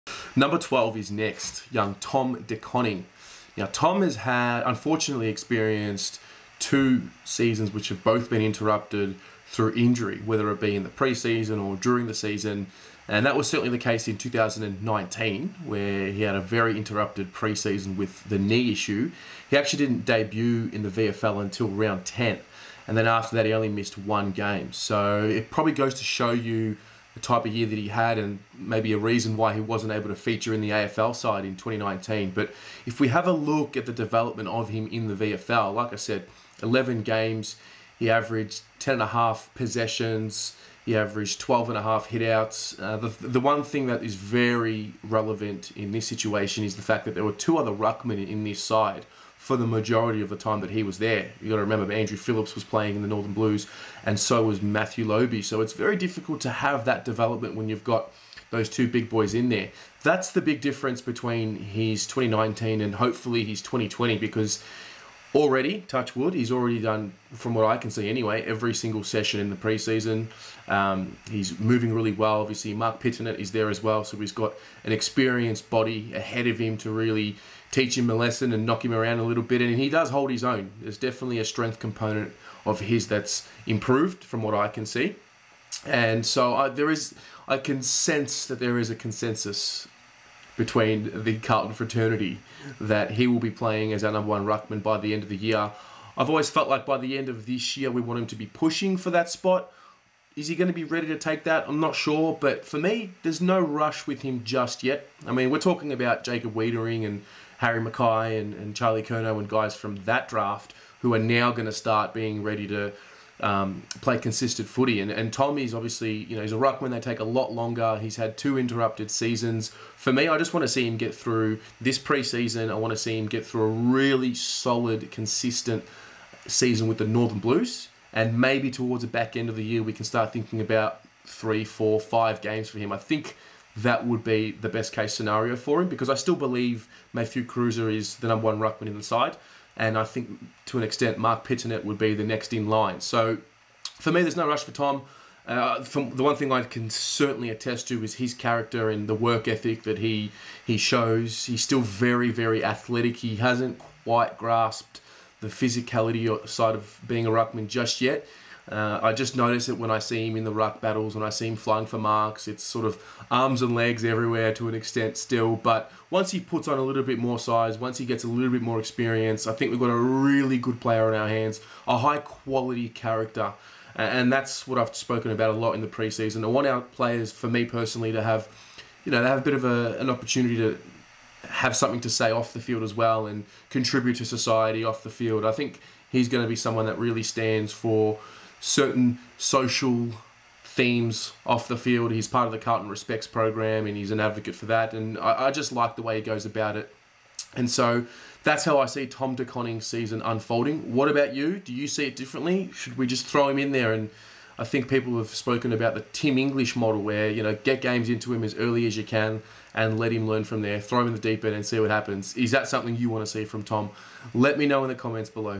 2020 Carlton Player Preview | Tom De Koning
Tom De Koning preview for the upcoming AFL 2020 season

Tom will be hoping to build off the 11 games he played for the Northern Blues in 2019 and take his development further so we can see how he competes at AFL level

More Carlton analysis, highlights and interviews

Go Blues!